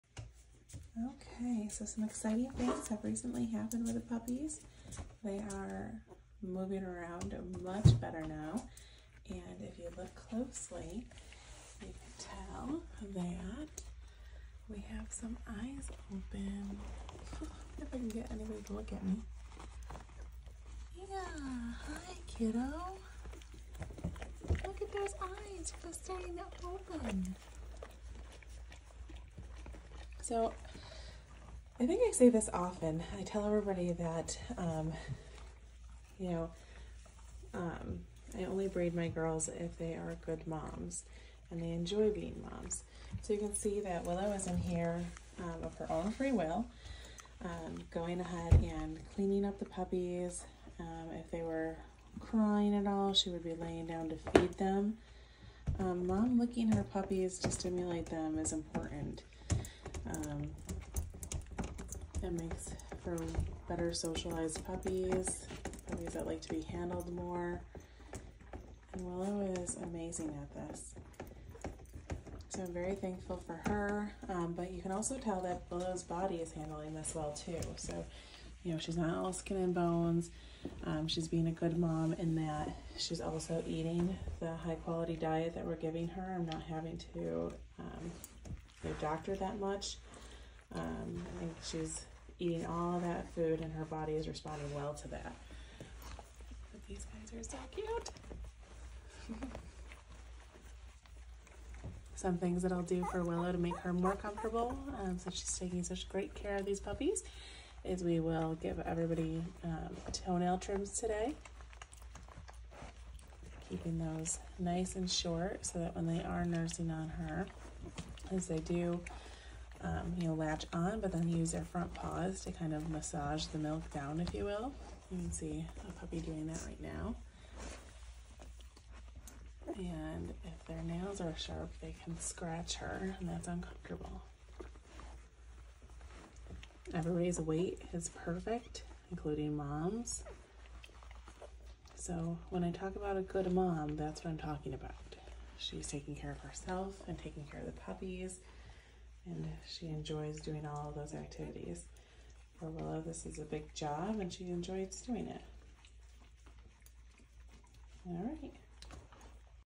Willow’s puppies @ 2 weeks old. Willow is a great mom and here is why.
What determines if a mom is a good mom? For puppies and my program it consists of a few things.
1. Mom has an uncomplicated whelping/delivery of pups

2. Mom is eager to care for the puppies.
       A. Puppies are getting enough milk
       B. Mom is cleaning the her puppies. This is important because as she licks them it awakens the glucocorticoid receptor gene. Puppies that are licked by mom or have a breeder that steps in when mom isn’t willing or able will grow up to be more relaxed in response to stress.

3. Mom is happy to eat a high quality diet and her body responds well to it. Meaning she maintains her own weight and an adequate milk supply for the puppies.